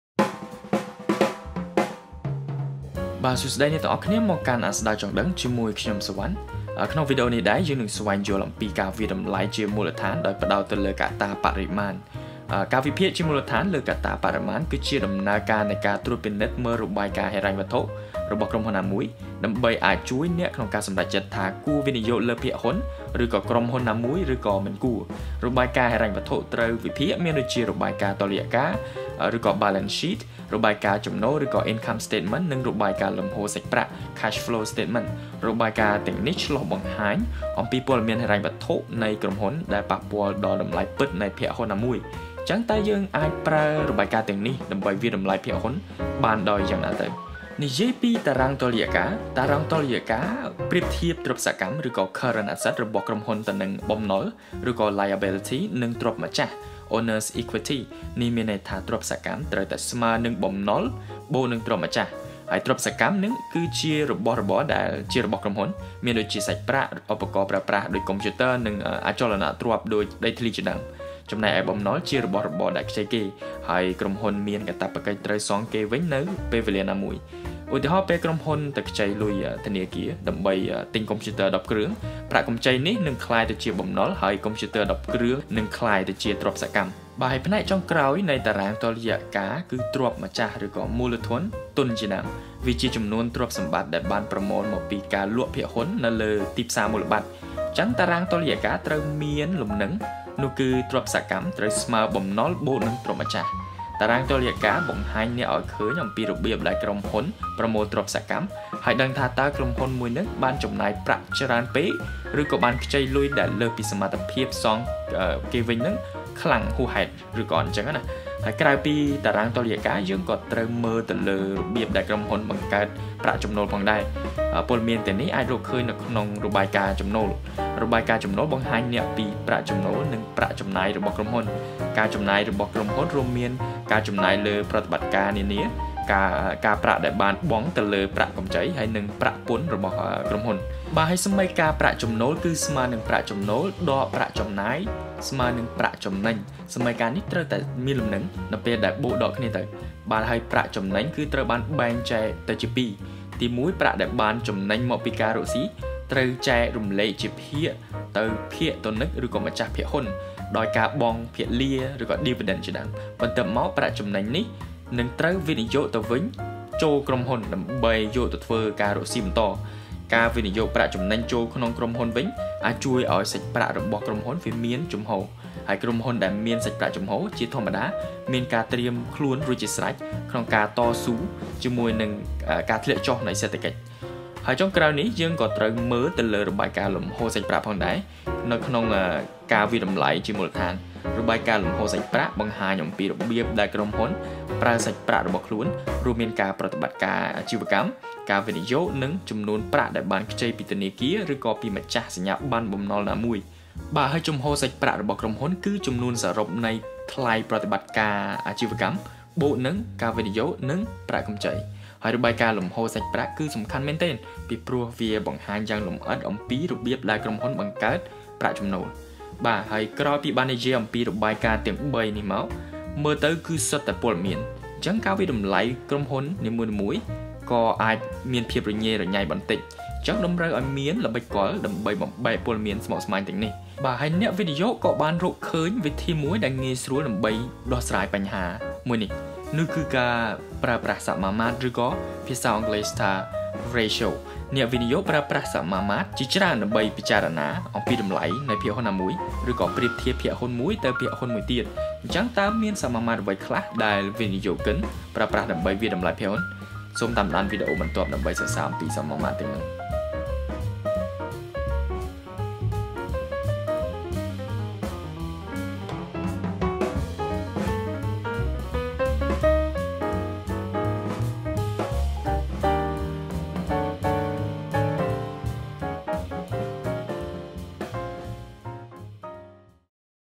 (វគ្គ៣) របៀបជ្រើសរើសភាគហ៊ុន [ការវិភាគកត្តាបរិមាណ?]
_របាយការណ៍ហិរញ្ញវត្ថុត្រូវវិភាគមានដូចជា (កត្តាបរិមាណ)
1. តារាងតុល្យការ(Balance Sheet)
2. របាយការណ៍ចំណូល(Income Statement)
3. របាយការណ៍លំហូរសាច់ប្រាក់(Cashflow Statement) ។